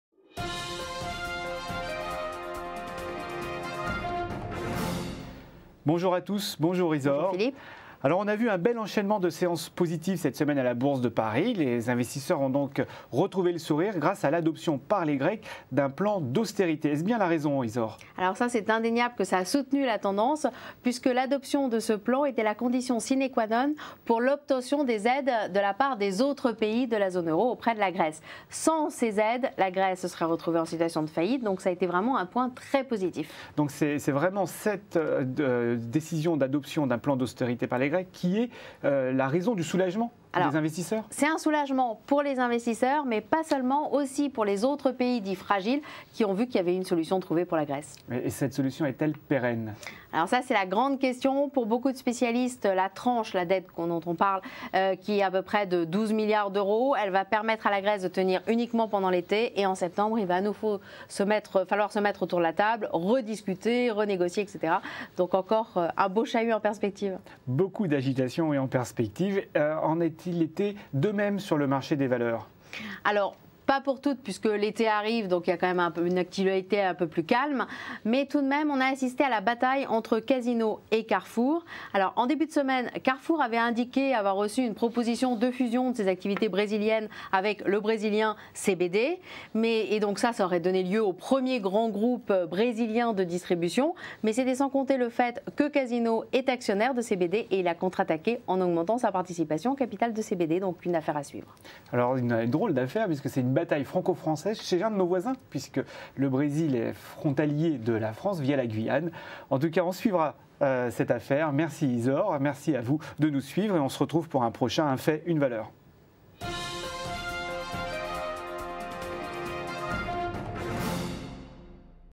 Incidence de l'adoption du plan d'austérité en Grèce
L'adoption du plan d'austérité en Grèce a soulagé les investisseurs. Mais le plan d'aide est-il suffisant ? Beaucoup de spécialistes répondent par la négative. Côté valeurs, Carrefour et Casino se disputent le marché brésilien